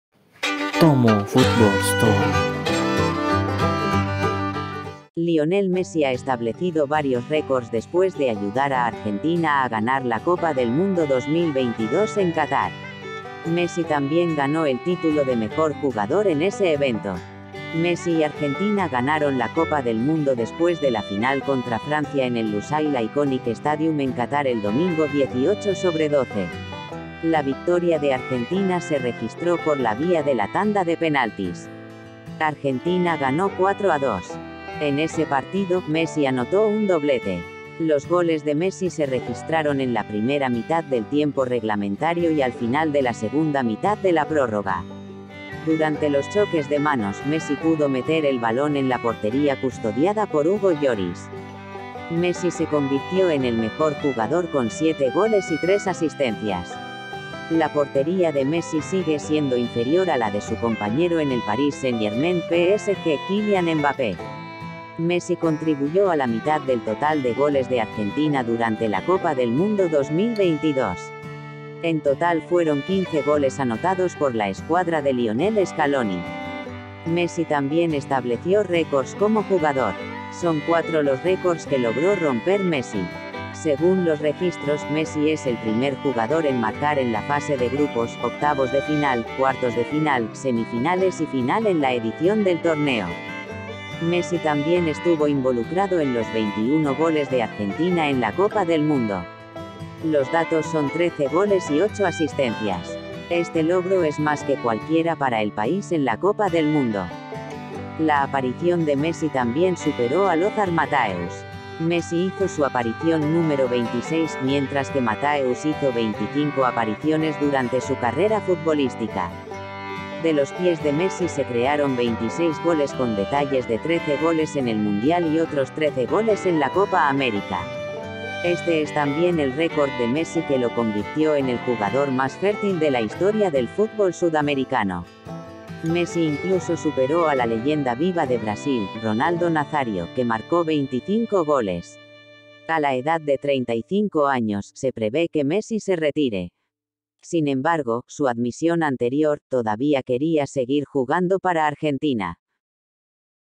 MESSI CONSIGUE CON ÉXITO 4 NUEVOS RÉCORDS - noticias DESTACADOS
Lionel Messi ha establecido varios récords después de ayudar a Argentina a ganar la Copa del Mundo 2022 en Qatar.  Messi también ganó el título de mejor jugador en ese evento.
  Messi y Argentina ganaron la Copa del Mundo después de la final contra Francia en el Lusail Iconic Stadium en Qatar el domingo (18/12).  La victoria de Argentina se registró por la vía de la tanda de penaltis.  Argentina ganó 4-2.
  En ese partido, Messi anotó un doblete.  Los goles de Messi se registraron en la primera mitad del tiempo reglamentario y al final de la segunda mitad de la prórroga.  Durante los choques de manos, Messi pudo meter el balón en la portería custodiada por Hugo Lloris.

  Messi se convirtió en el mejor jugador con 7 goles y 3 asistencias.  La portería de Messi sigue siendo inferior a la de su compañero en el Paris Saint-Germain (PSG) Kylian Mbappé.

  Messi contribuyó a la mitad del total de goles de Argentina durante la Copa del Mundo 2022. En total fueron 15 goles anotados por la escuadra de Lionel Scaloni.

  Messi también estableció récords como jugador.  Son cuatro los récords que logró romper Messi.

  Según los registros, Messi es el primer jugador en marcar en la fase de grupos, octavos de final, cuartos de final, semifinales y final en la edición del torneo.

  Messi también estuvo involucrado en los 21 goles de Argentina en la Copa del Mundo.  Los datos son 13 goles y 8 asistencias.  Este logro es más que cualquiera para el país en la Copa del Mundo.
  La aparición de Messi también superó a Lothar Matthaeus.  Messi hizo su aparición número 26, mientras que Matthaeus hizo 25 apariciones durante su carrera futbolística.

  De los pies de Messi se crearon 26 goles con detalles de 13 goles en el Mundial y otros 13 goles en la Copa América.  Este es también el récord de Messi que lo convirtió en el jugador más fértil de la historia del fútbol sudamericano.  Messi incluso superó a la leyenda viva de Brasil, Ronaldo Nazario, que marcó 25 goles.

  A la edad de 35 años, se prevé que Messi se retire.  Sin embargo, su admisión anterior, todavía quería seguir jugando para Argentina.